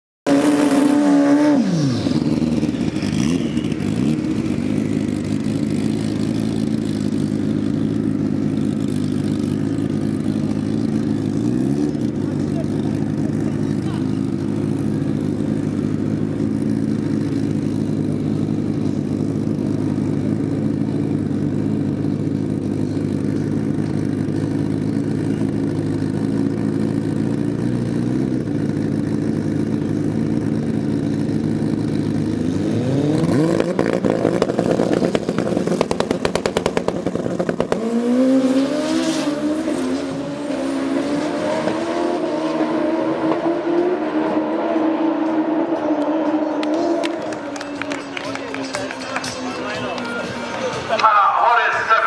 May 2015 QTM Day 3 Run 90
3rd May 2015 - Day 3 "Eliminations and Finals" of the 2nd Quarter Mile Event for 2014, Hosted by the MDRA at Hal Far Raceway.